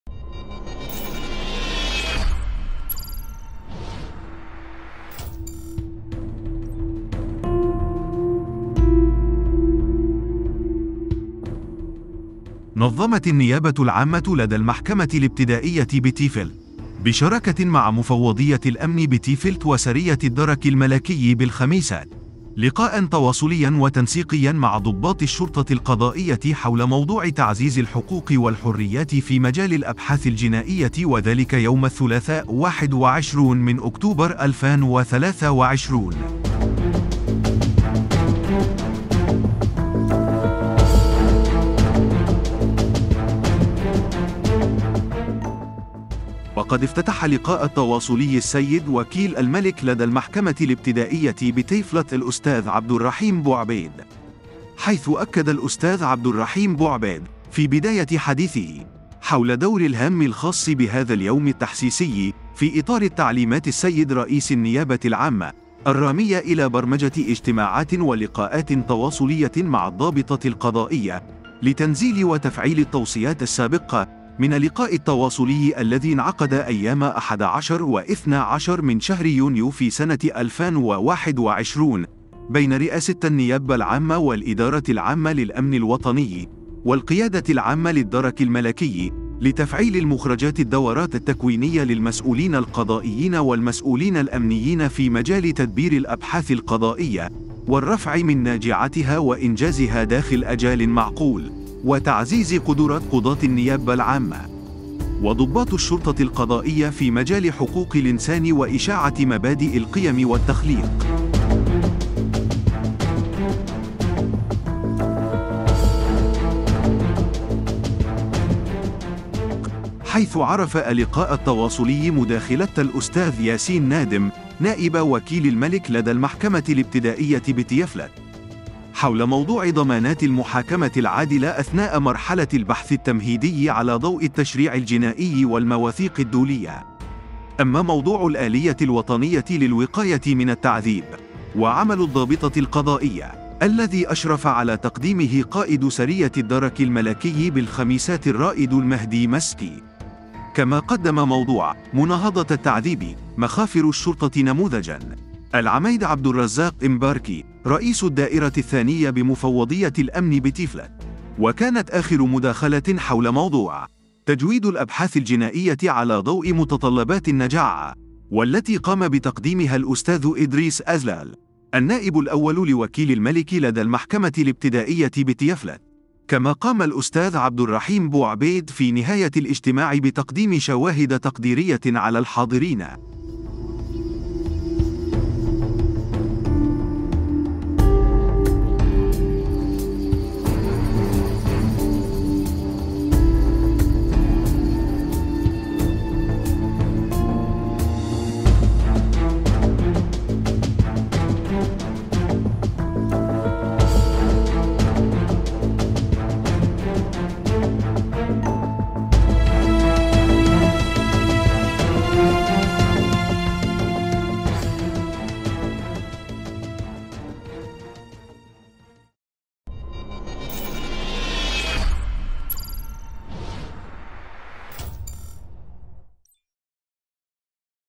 المحكمة الابتدائية بتيفلت تنظم لقاءا تواصلياً بشراكة مع الأمن الوطني والدرك الملكي
نظمت النيابة العامة لدى المحكمة الابتدائية بتيفلت بشراكة مع مفوضية الأمن بتيفلت وسرية الدرك الملكي بالخميسات لقاءا تواصليا وتنسيقيا مع ضباط الشرطة القضائية حول موضوع تعزيز الحقوق والحريات في مجال الأبحاث الجنائية، وذلك يوم الثلاثاء 21 من أكتوبر 2023.

وقد افتتح للقاء التواصلي السيد وكيل الملك لدى المحكمة الابتدائية  بتيفلت الأستاذ عبد الرحيم بوعبيد.

حيث أكد السيد عبد الرحيم بو عبيد في بداية حديثه حول دور الهام لليوم التحسيسي في إطار التعليمات السيد رئيس النيابة العامة الرامية إلى برمجة اجتماعات وللقاءات تواصلية مع الضابطة القضائية لتنزيل وتفعيل التوصيات للقاء التواصلي الذي انعقد أيام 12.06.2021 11
 بين رئاسة النيابة العامة والإدارة العامة للأمن الوطني والقيادة العامة للدرك الملكي لتفعيل المخرجات الدورات التكوينية  للمسؤولين القضائيين والمسؤولين الأمنيين في مجال تدبير الأبحاث القضائية والرفع من ناجعتها وانجازها داخل أجال معقول وتعزيز القدرات قضاة النيابة العامة وضباط الشرطة القضائية في مجال حقوق الانسان وإشاعة مبادئ القيم والتخليق.

حيث عرف للقاء التواصلي مداخلة الأستاذ ياسين نادم، نائب وكيل الملك لدى المحكمة الابتدائية بتيفلت. حول موضوع ضمانات المحاكمة العادلة أثناء مرحلة البحث التمهيدي على ضوء التشريع الجنائي والمواثيق الدولية.

أما موضوع الآلية الوطنية للوقاية من التعذيب وعمل الضابطة القضائية، الذي أشرف على تقديمه قائد سرية الدرك الملكي بالخميسات الرائد المهدي مسكي.

كما قدم موضوع، مناهضة التعذيب، مخافر الشرطة نموذجا.. العميد عبد الرزاق امباركي رئيس الدئرة الثانية بمفوضية الأمن بتيفلت.

وكانت آخر مداخلة حول موضوع تجويد الأبحاث الجنائية على ضوء متطلبات النجاعة.. والتي قام بتقديمها الأستاذ\ ادريس أزلال، النائب الأول لوكيل الملك لدى المحكمة الابتدائية بتيفلت.

كما قام الأستاذ عبد الرحيم بوعبيد في نهاية للقاء بتقديم شواهد تقديرية على الحاضرين.